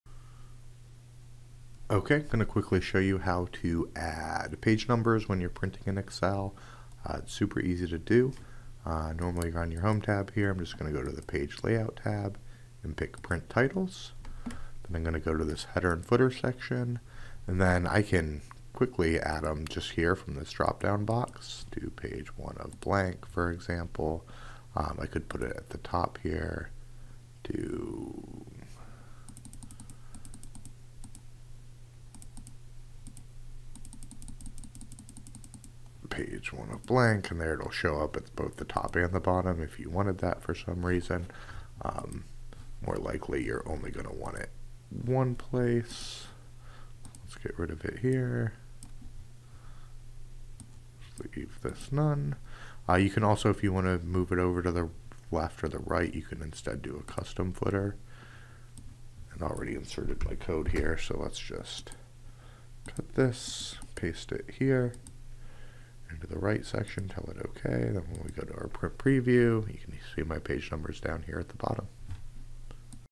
How to Print Page Numbers in Excel
Brief video lesson on how to print page numbers in Excel.